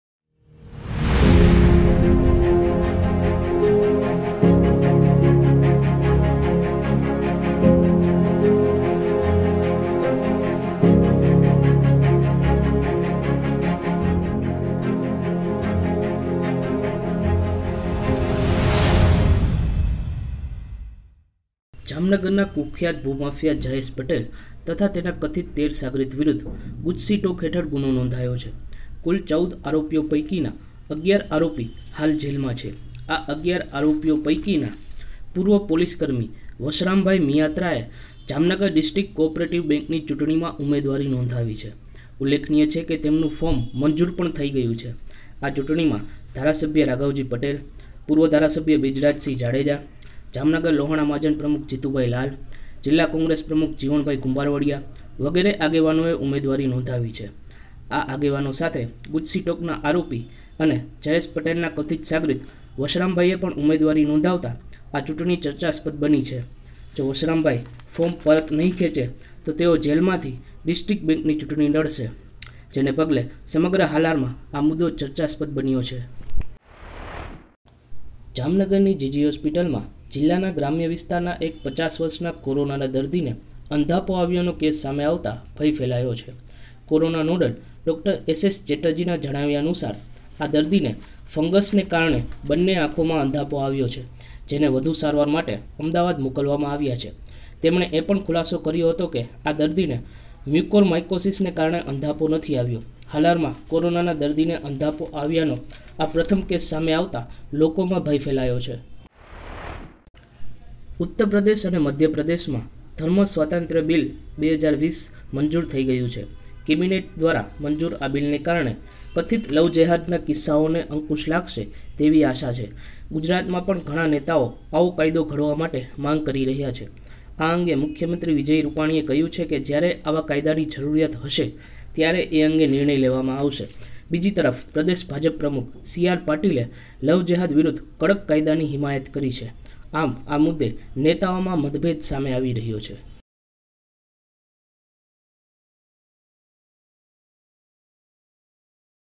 News Flash 26-12-2020 : ભૂમાફિયાનાં કથિત સાગરિત પૂર્વ પોલીસકર્મી જેલમાંથી ચૂંટણી લડશે
Reader Aditya Jamnagari